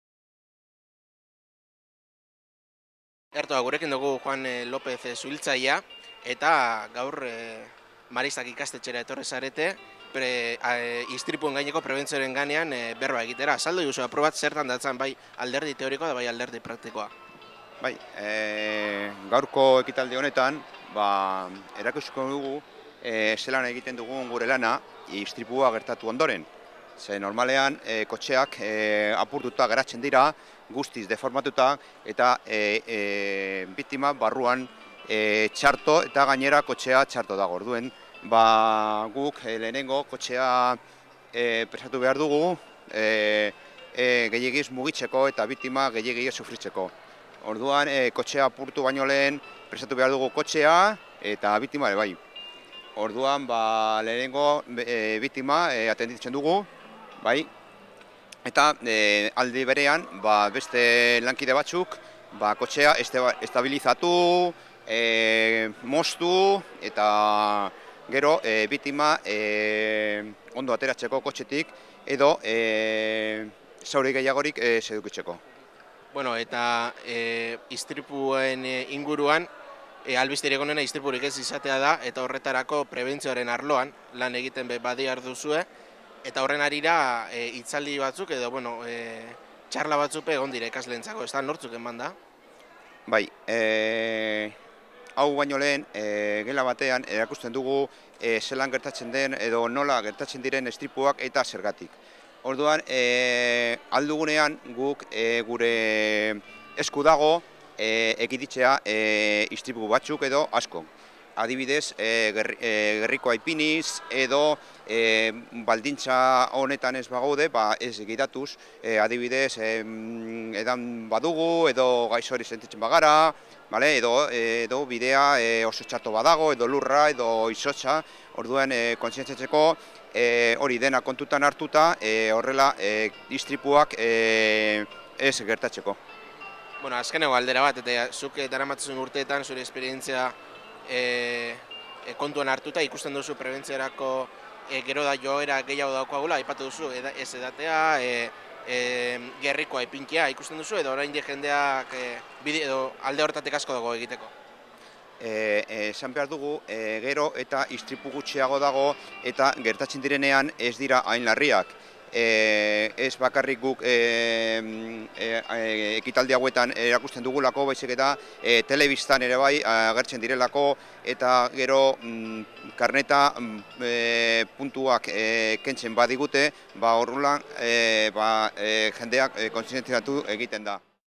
Simulakroa Maristetan.mp4
Stop Accidentes elkarteak eta su-hiltzaileek auto istripuen prebentziorako erakustadia eskaini dute Durangoko Maristak ikastetxean